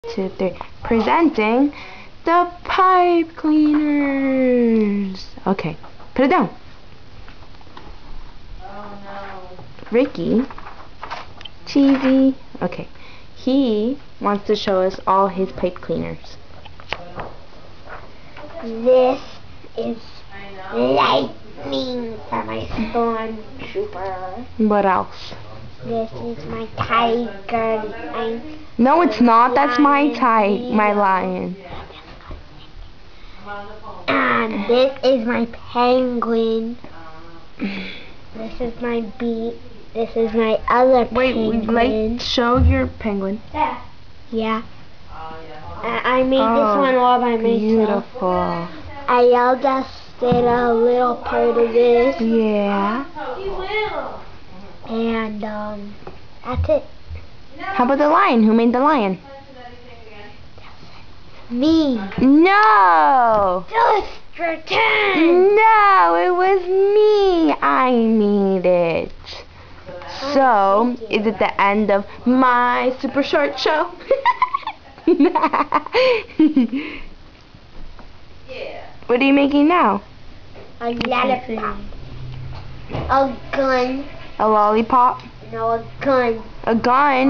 Pipe cleaners movie
Ricky shows off his pipe cleaners